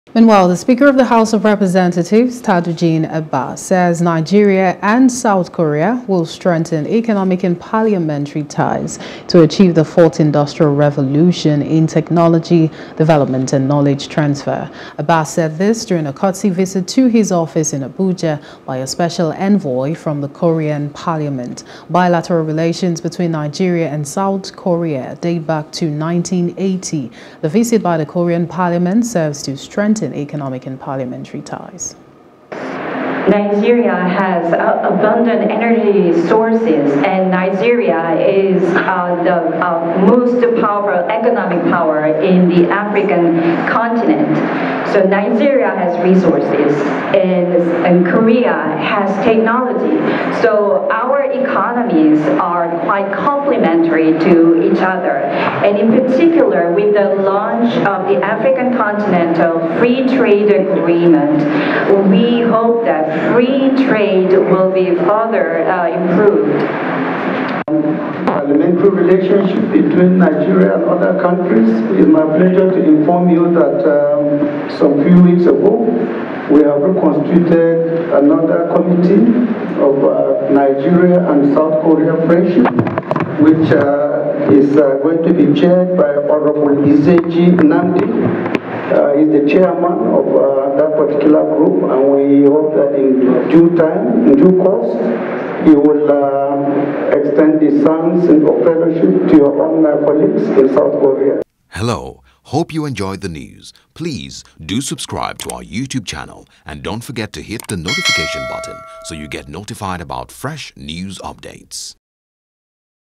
Courtesy Visit: Speaker Abbas Receives Korean Delegation
The speaker of the house of representatives Rt. Hon. Tajudeen Abbas says Nigeria and South Korea will strengthen economic and parliamentary ties to achieve the fourth industrial revolution in technology, development and knowledge transfe